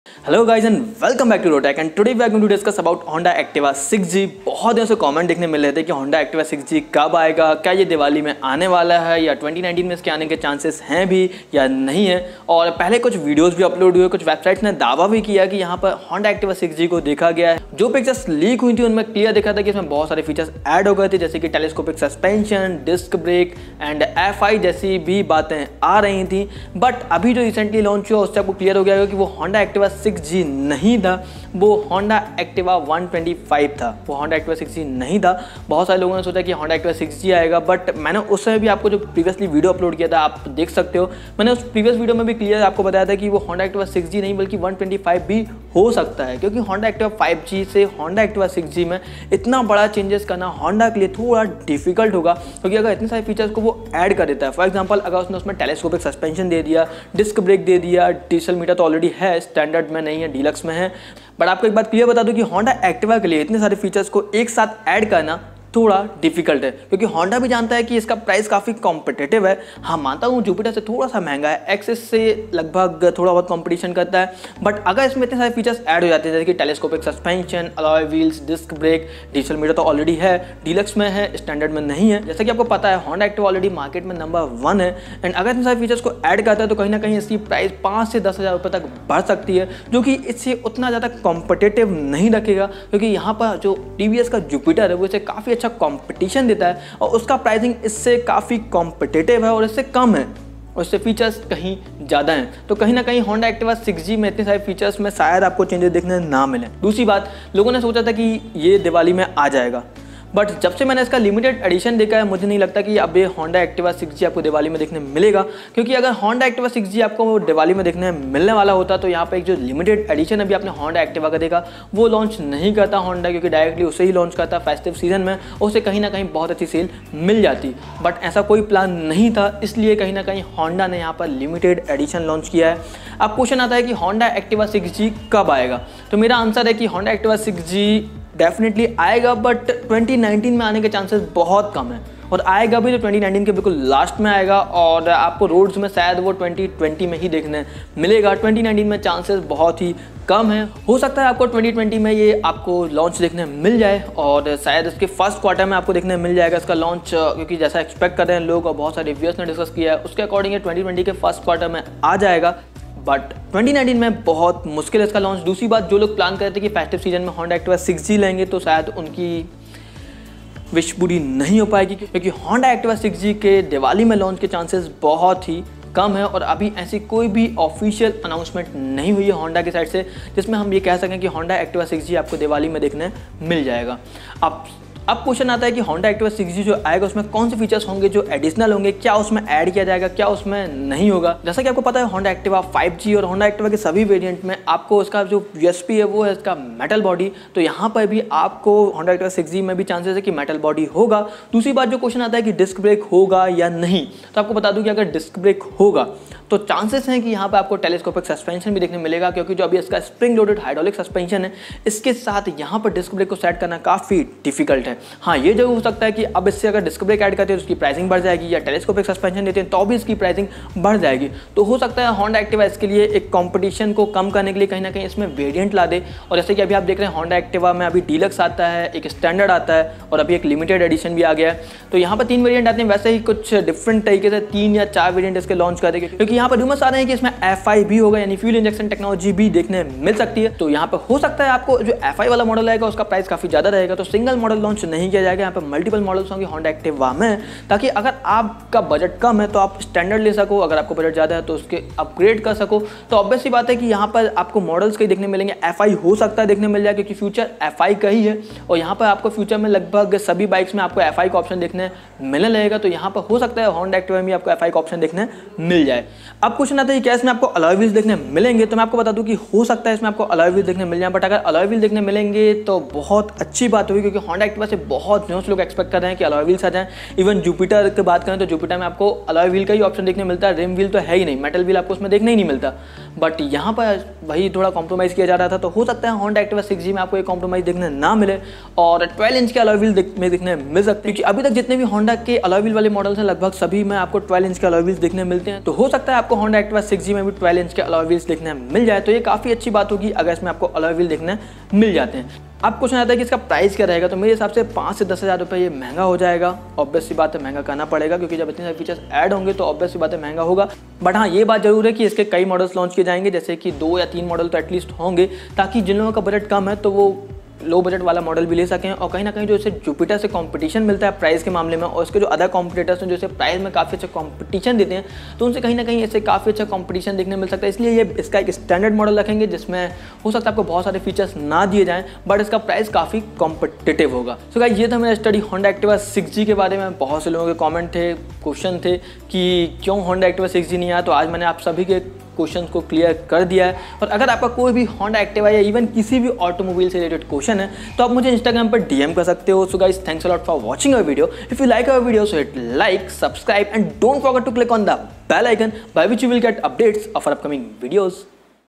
Activa 6G कब आएगा... Price, Engine Specification, features, Launch date, Competition
My Mic
My Phone (One Plus 6T)
My Laptop
My Helmet
My Tripod
Hello guys and welcome back to ROTAQ, which means Reviews of Technology & Query, this is Pratool Dubey. Today we are going to discuss about Honda Activa 6G.
Honda is all set to launch a new BS6 two-wheeler on June 12. The company hasn’t specified whether it’s going to be a motorcycle or a scooter, but we expect the upcoming offering to be the latter. This is because a test mule of the same was spotted outside the ARAI facility recently. It was unclear as to which Activa it was but the body panels and the instrument cluster indicate that it could be the Activa 125. However, we have to wait till the official launch for more details. As per online reports, Honda is set to equip the upcoming Activa 6G with a host of features. This includes smartphone connectivity, telescopic forks, 12-inch alloy wheels and most importantly a fuel-injected BSVI-compliant engine. Safe to say, this can add a premium of Rs 5,000 to Rs 7,000 to its current pricing. We expect Honda do debut the updated scooter at the 2020 Auto Expo.